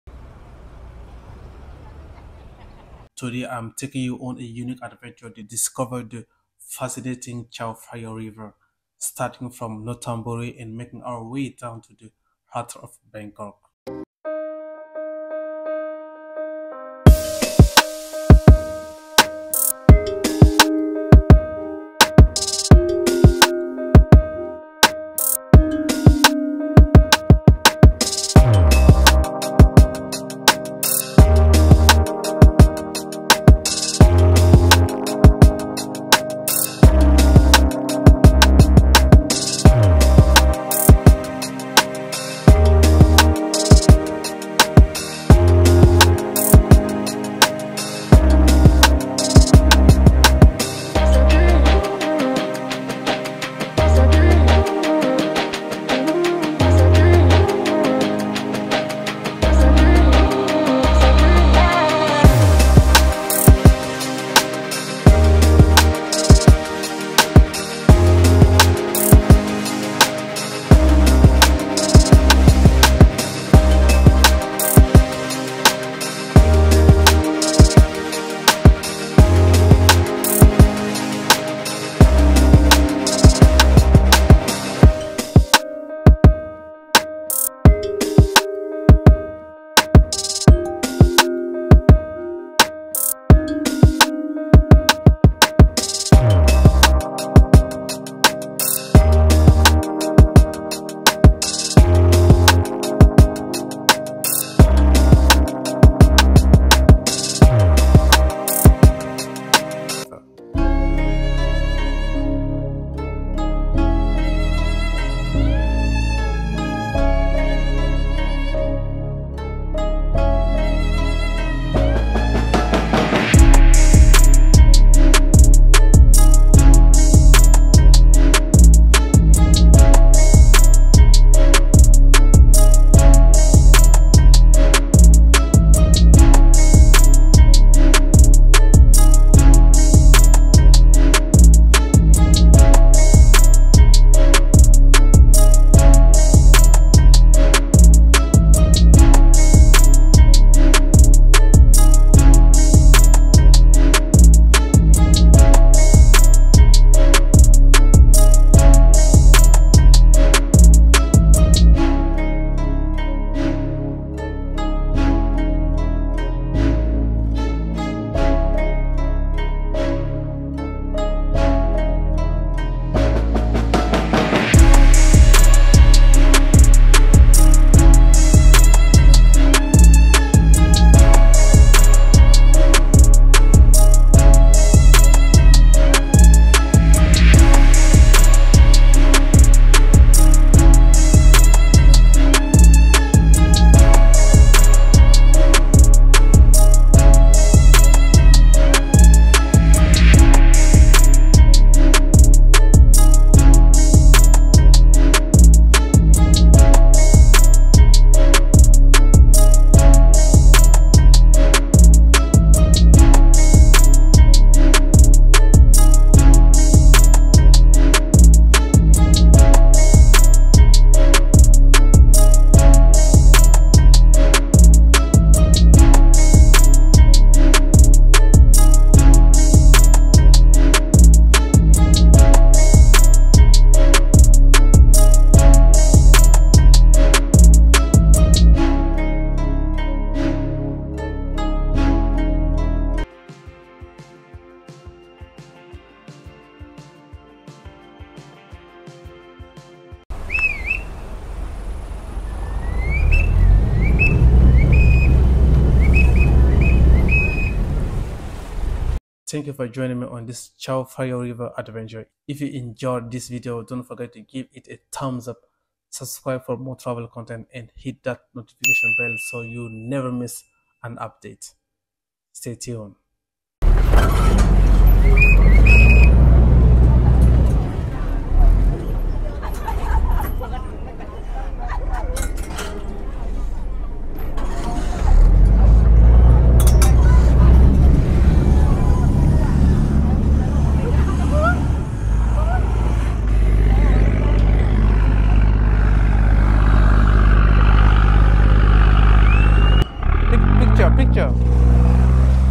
You Won’t Believe! Cheapest Boat Cruise in Bangkok Thailand
Hey, in this video, I take you on a very affordable river trip in Bangkok.

If you’d like to help the channel grow and would like to donate, you can do so here: